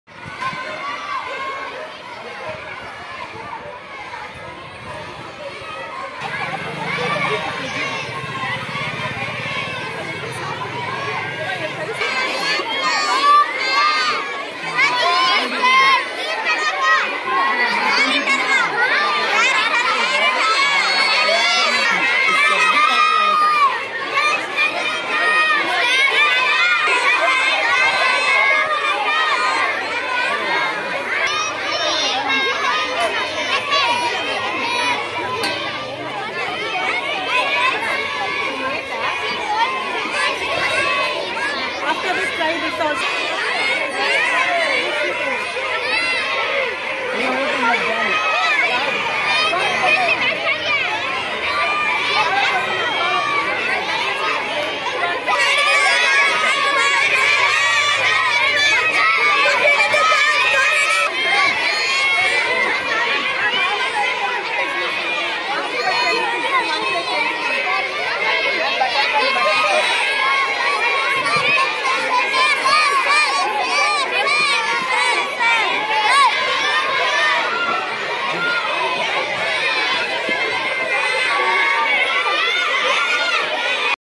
abhyas mela 😊😊 .... fresh fruits ,veggies, stationery, beauty , foods available hear ....
everything thing was available here 😀